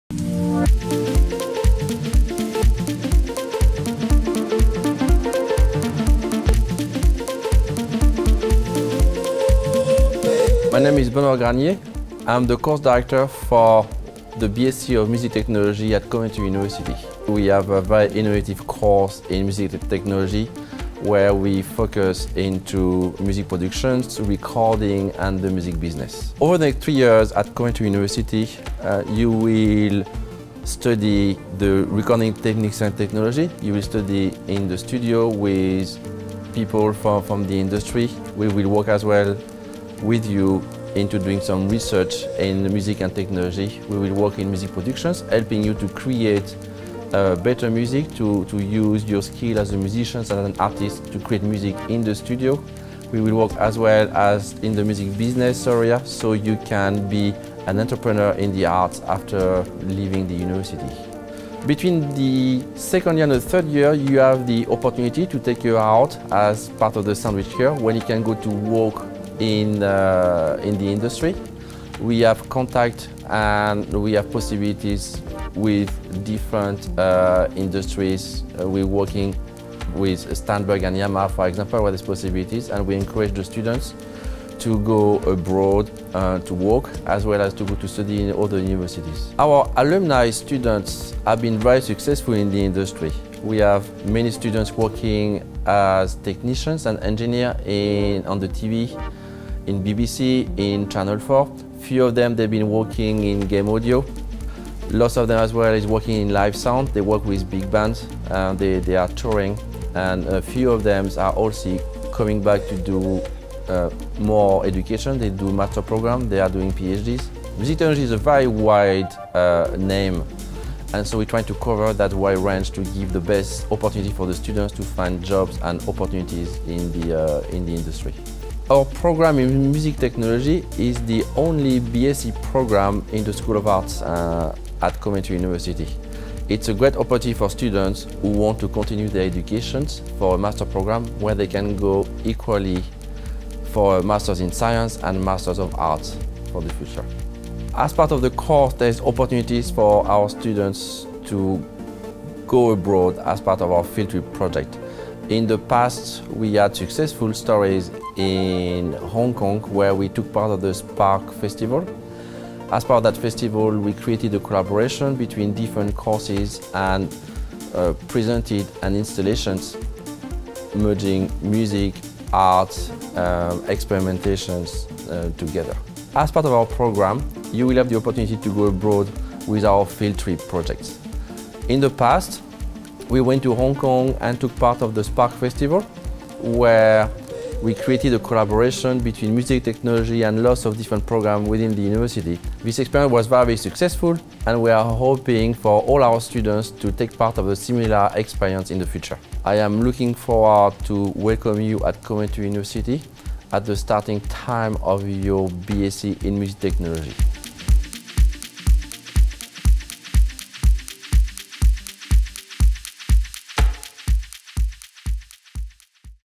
Study Music Technology (BSc) at Coventry University
Join course director Benoit Granier on a tour and see what it means to study music technology (BSc) in the faculty of Arts and Humanities at Coventry University, UK.